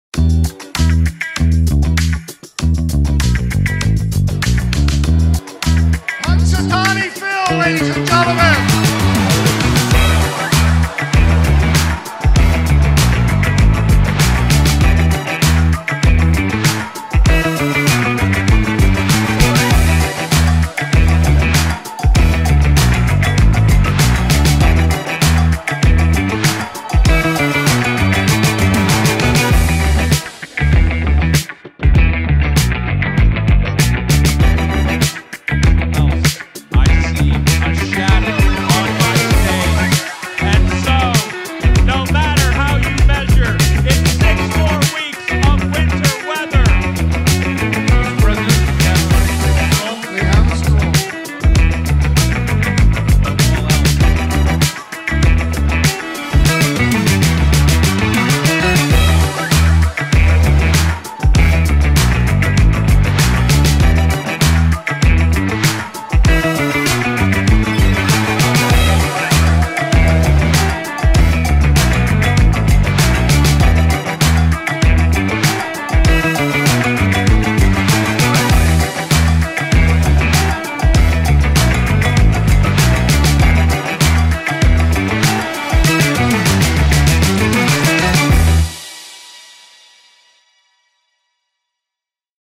ينتظره الملايين سنوياً.. "جرذ الطقس" يخرج من جحره في أميركا ويثير انقساماً عن نهاية فصل الشتاء
"جرذ الطقس" يخرج من جحره في أميركا ويثير انقساماً كبيراً بشأن موعد نهاية فصل الشتاء: ما قصة الجرذ الذي ينتظره ملايين الأميركيين؟